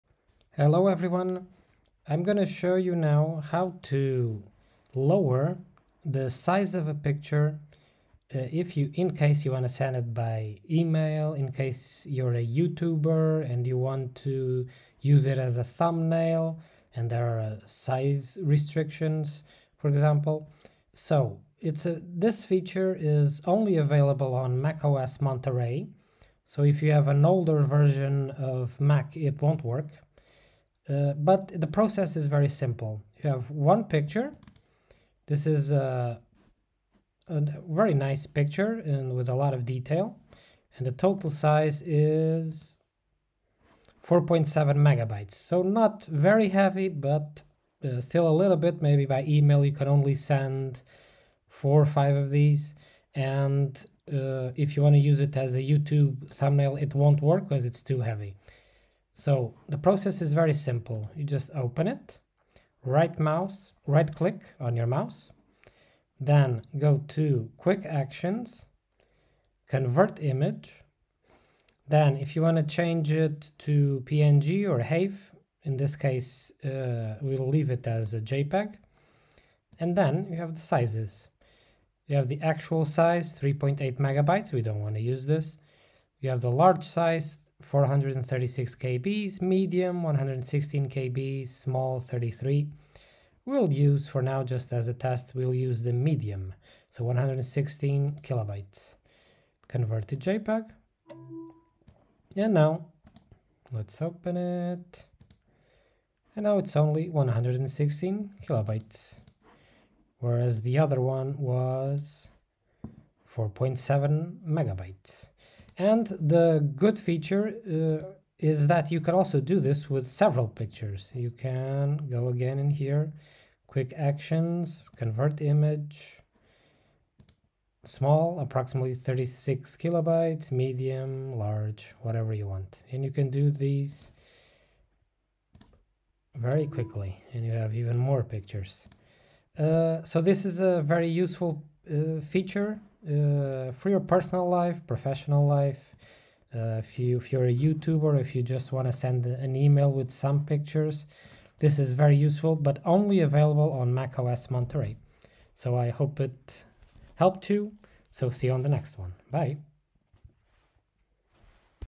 How to reduce Image size on a Mac (VERY FAST!)
This is a video explaining how to reduce an image or picture size on your Mac (only available on Mac OS Monterey).